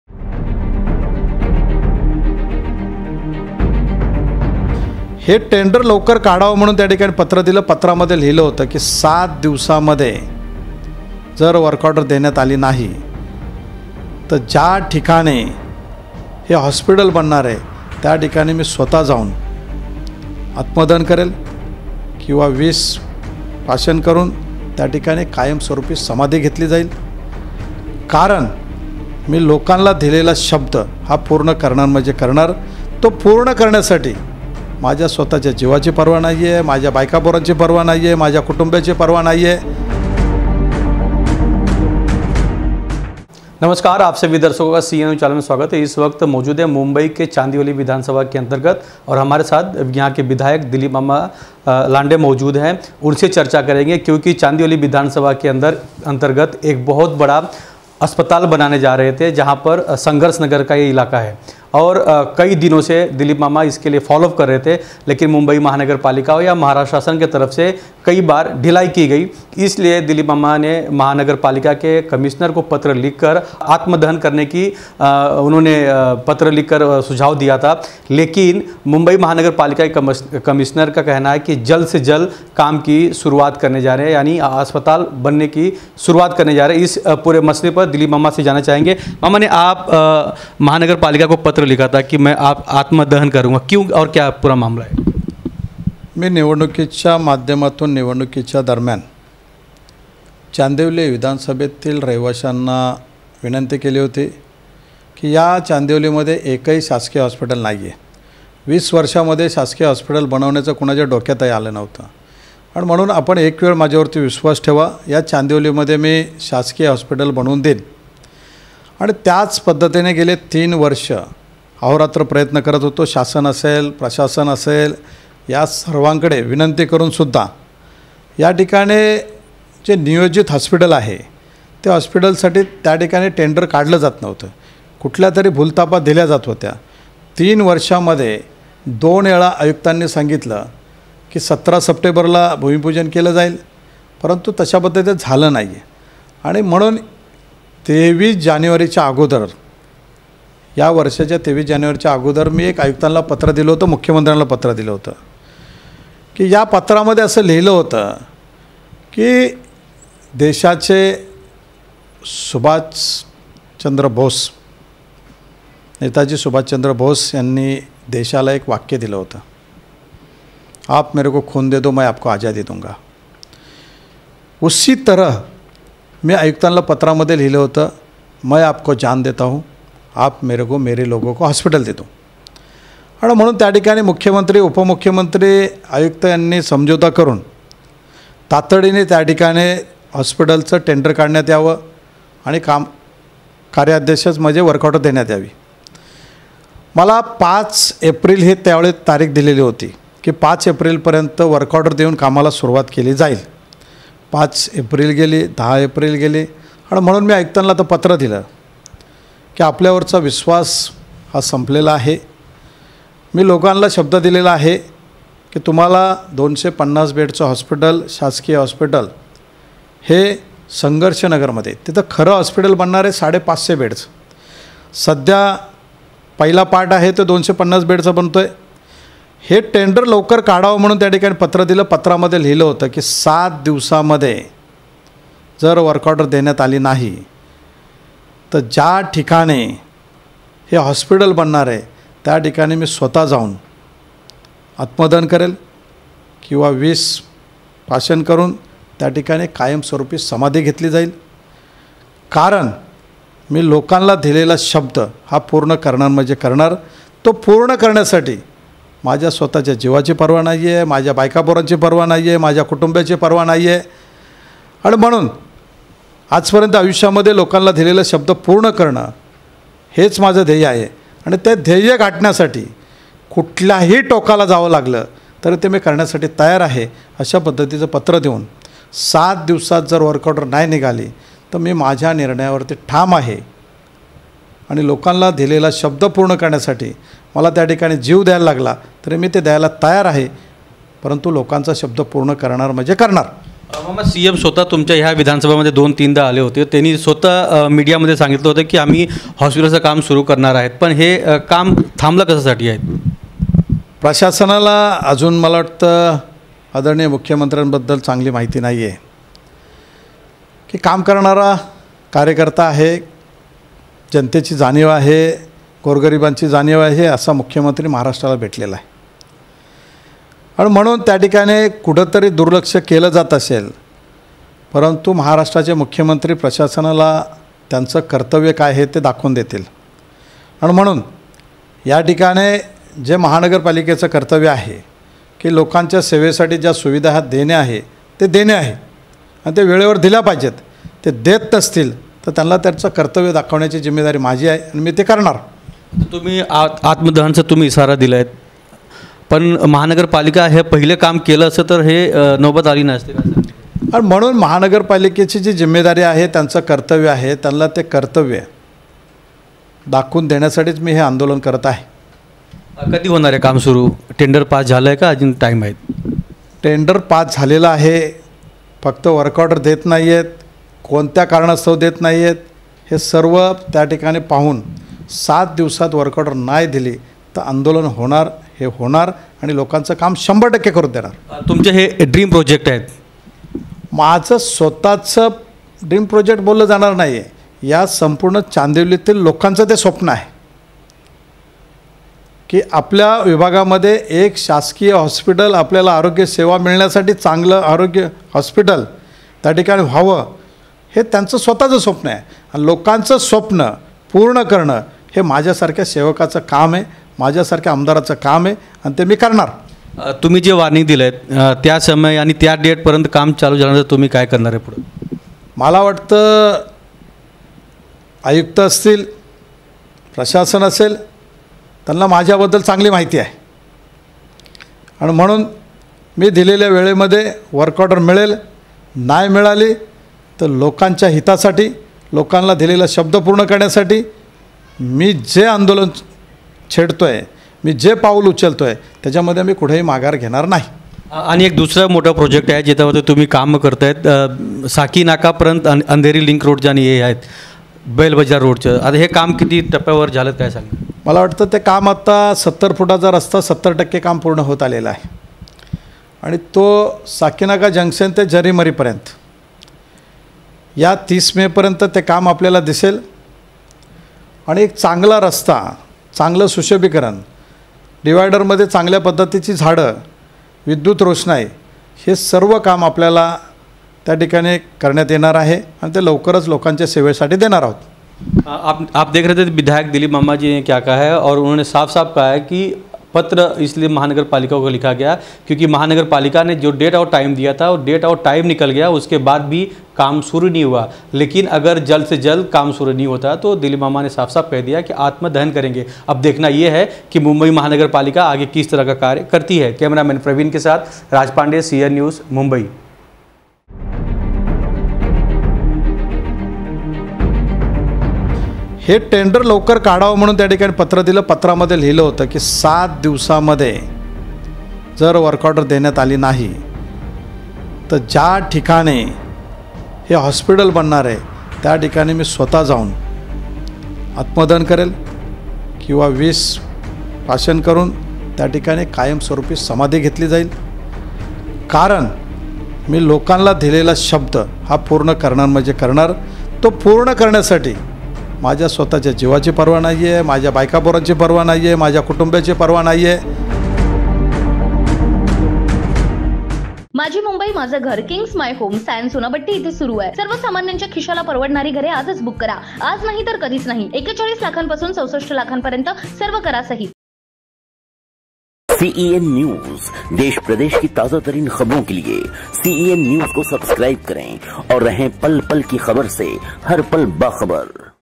चांदिवली विधानसभा में अस्पताल नही बना तो शिवसेना विधायक दिलीप मामा लांडे करेंगे आत्मदहन, दी चेतावनी
DILIP MAMA LANDE || CEN NEWS || MUMBAI ||